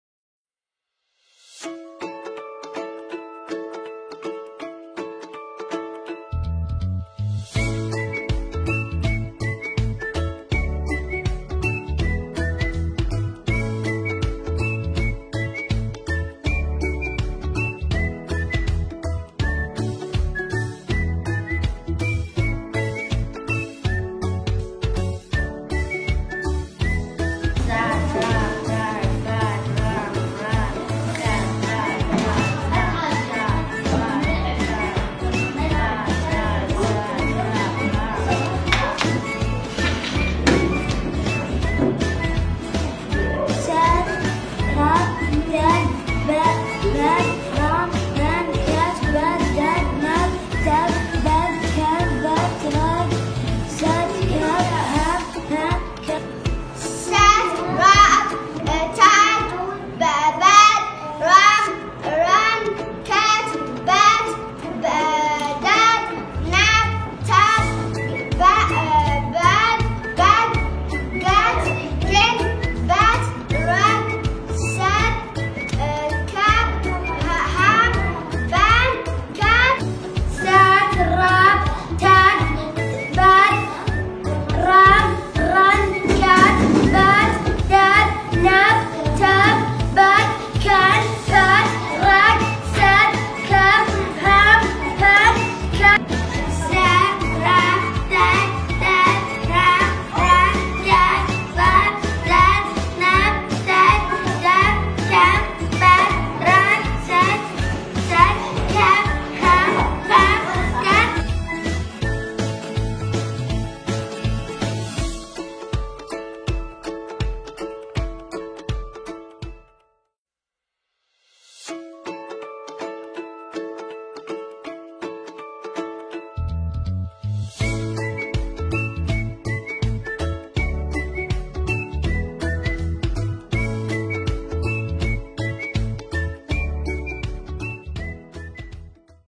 Reema_International_School 📖Spinner Reading Activity📖
🌹Transitional Section🌹
Prior to encouraging a child to practice reading, it is necessary to awaken motivation and curiosity using playing that interests the student🌹
📖Spinner Reading Activity📖
Transitional class 3

T.Hina